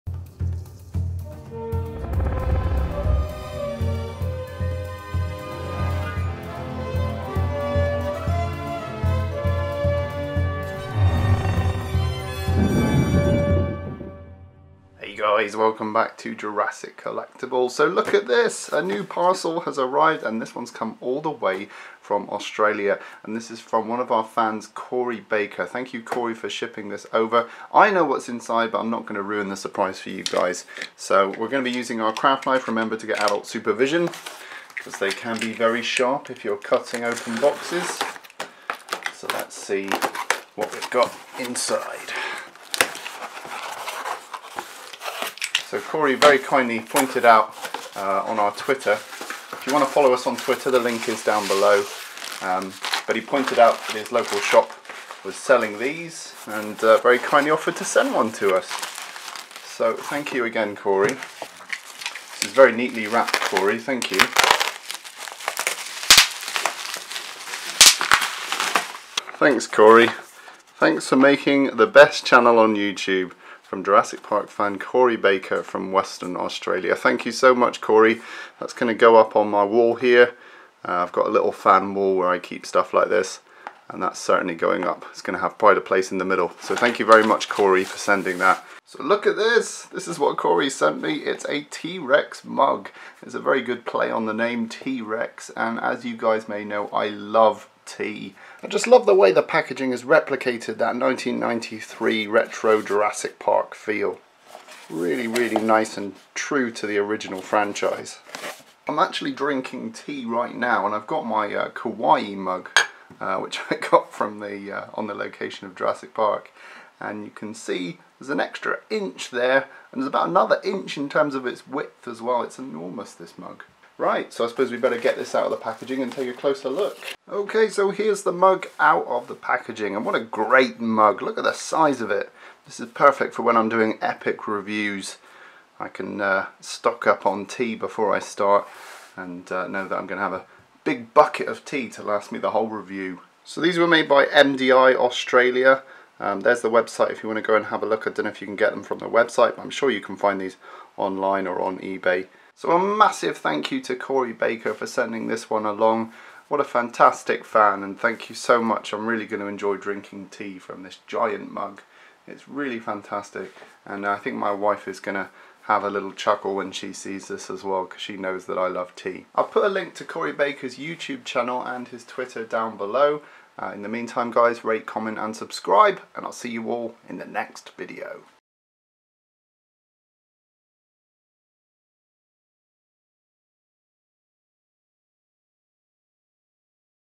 Fan Parcel Unboxing!
We were very kindly sent a parcel all the way from Western Australia by Corey Baker, one of our long time fans of the channel.  Thanks Corey!